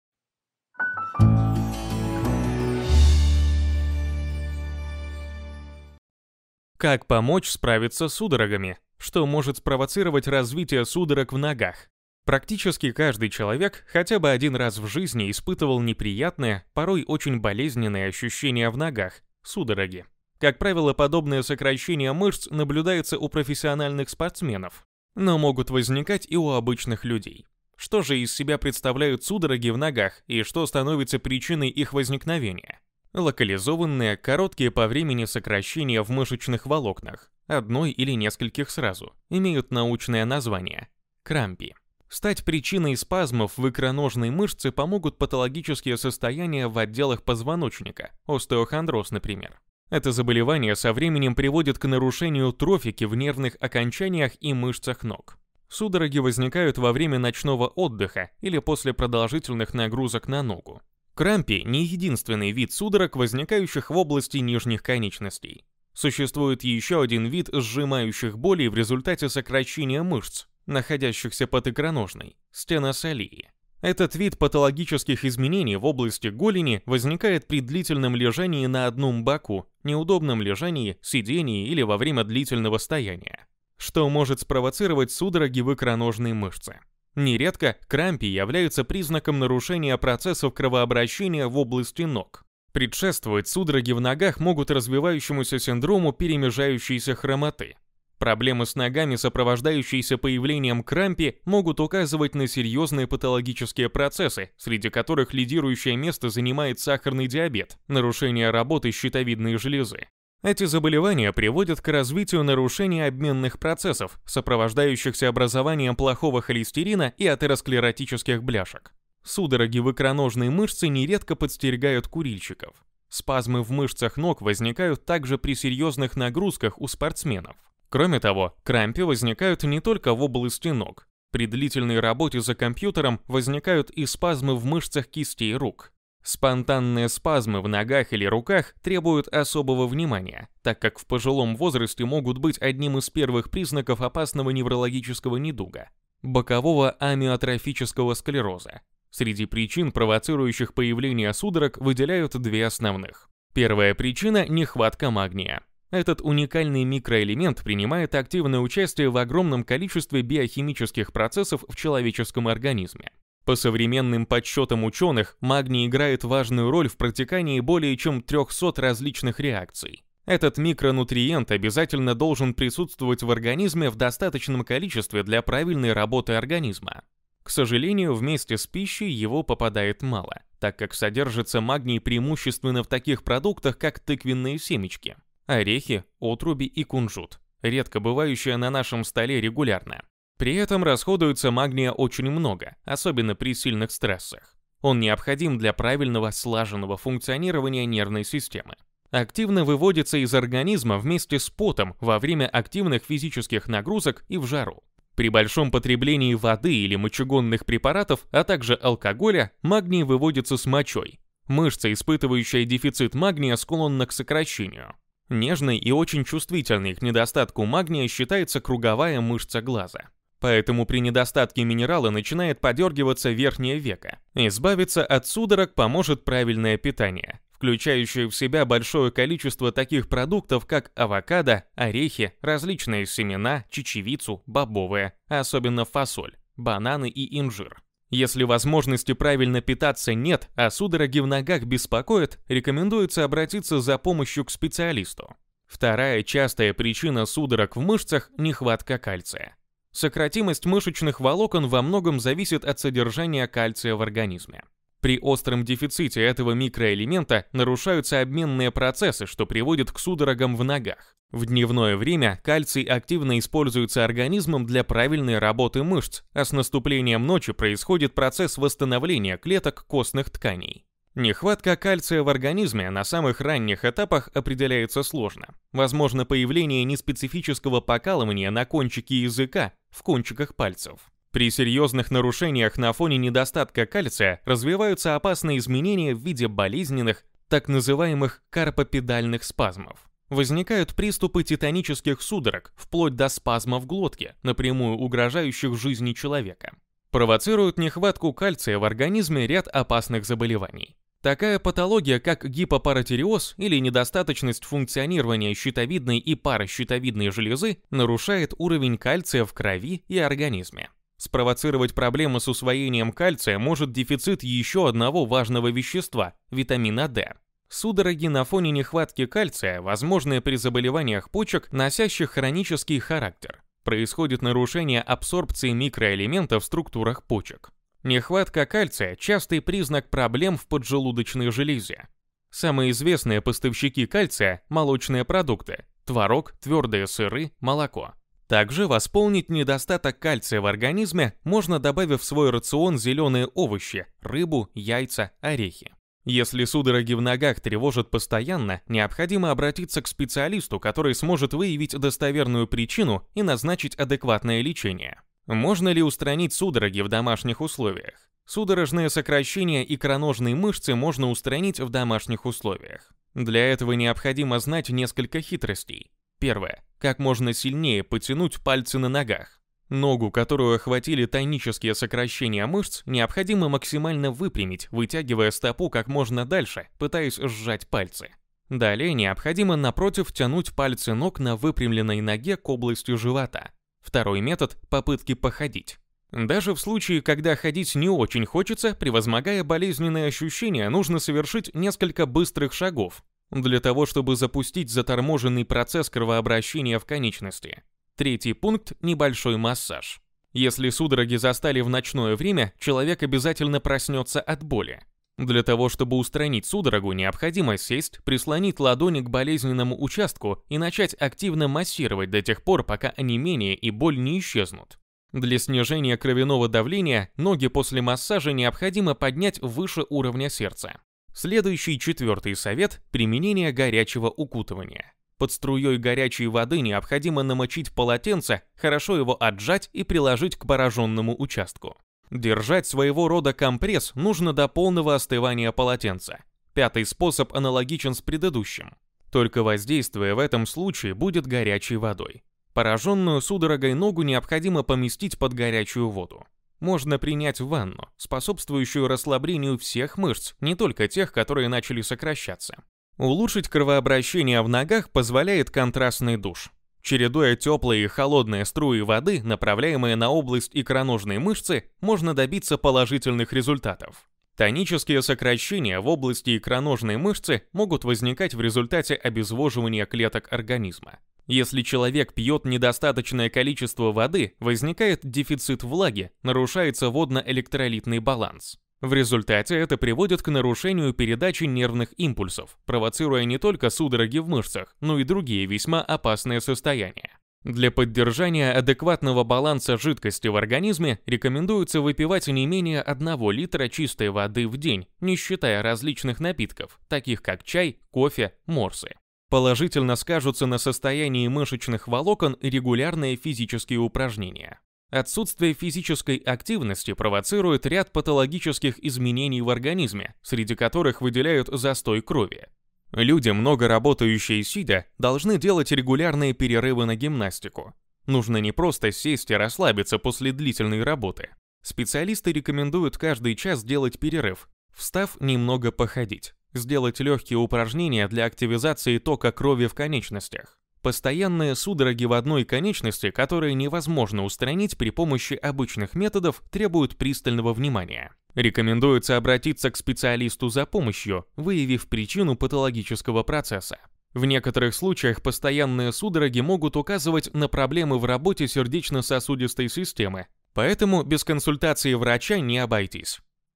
Сводит ноги? Как избавиться? Вот причины и простые способы лечения
Как помочь справится с судорогами? Что может спровоцировать развитие судорог в ногах?
Практически каждый человек, хотя одни раз и жизни испытывал неприятные, порой очень болезненные ощущения в ногах – судороги. Как правило, подобные сокращения мышц, наблюдаются у профессиональных спортсменов, но могут возникать и обычных людей.
Что же из себя представляют судороги в ногах и что становиться причиной их возникновения?

➤➤➤О НАС:
Канал посвящен здоровью:

➤➤➤ВНИМАНИЕ!